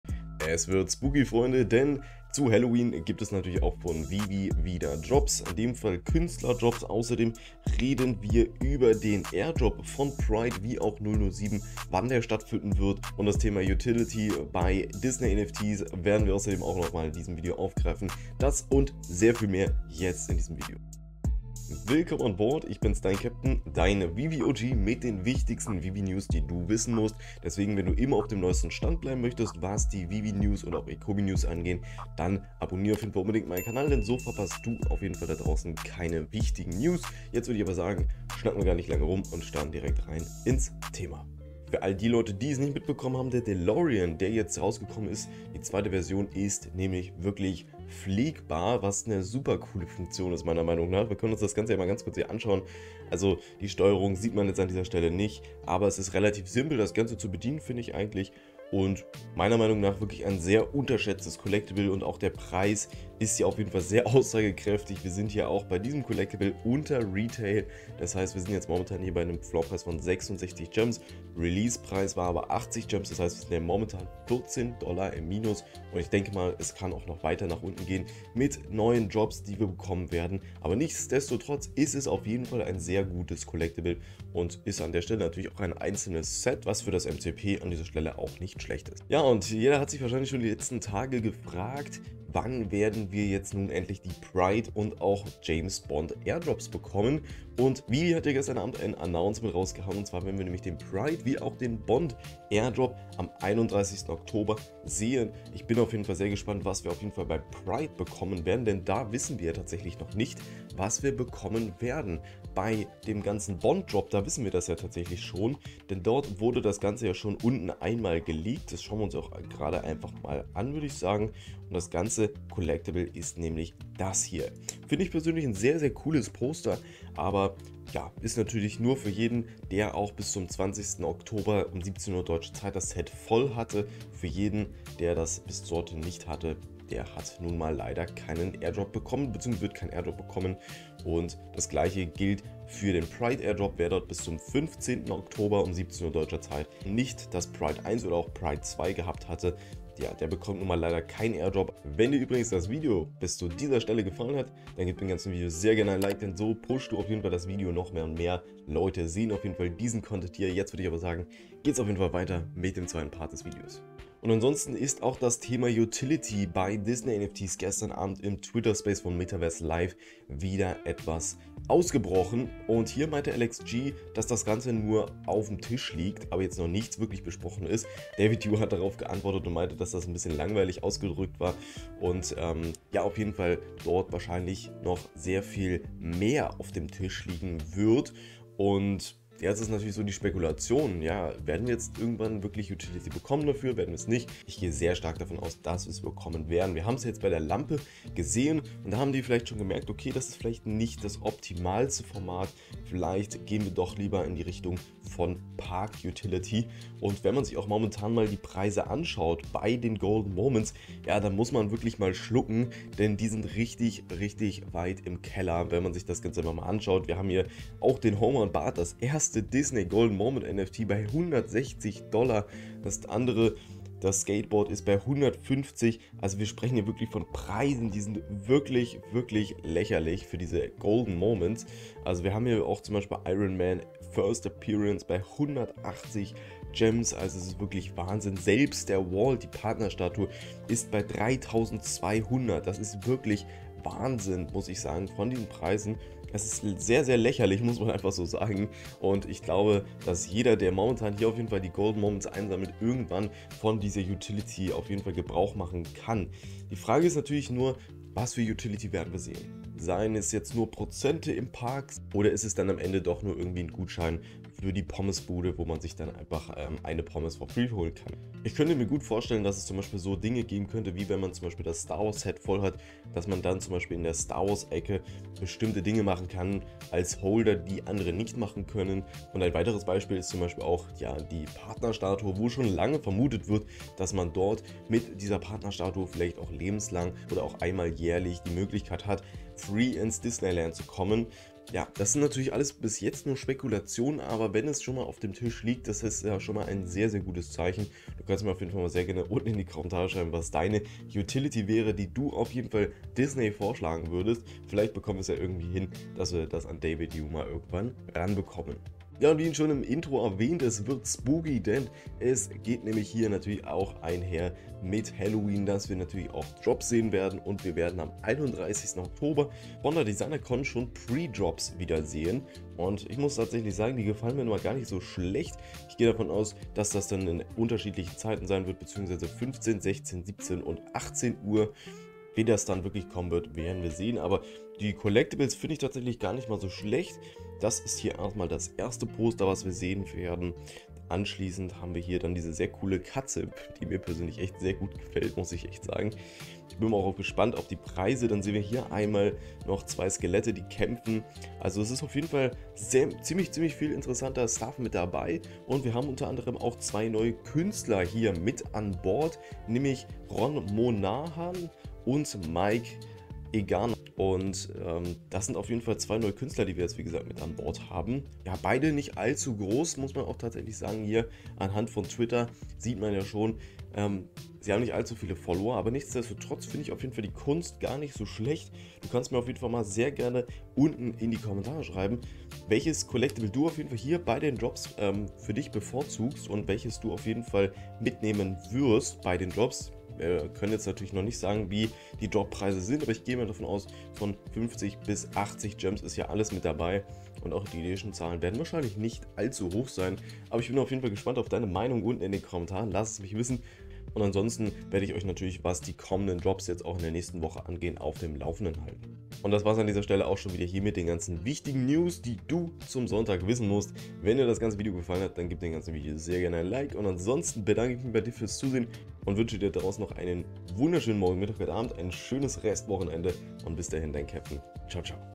Helloween VeVe NFT Drops! | AirDrops auf VeVe am ... | VeVe News
Zu Helloween gibt es von der Designercon 4 NFT Drops auf VeVe. Außerden ist bekannt gegeben worden, wann die zwei ausstehenden AirDrops auf VeVe statt finden.

                  ➖➖➖➖➖➖⬇️Quellen⬇️➖➖➖➖➖